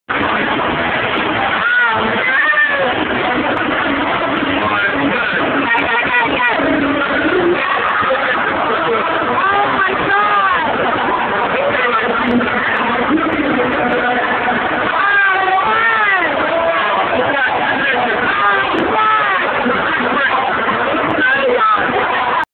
Shadow Pulling Midget
Shadow pulling a midget in Oceana in 2006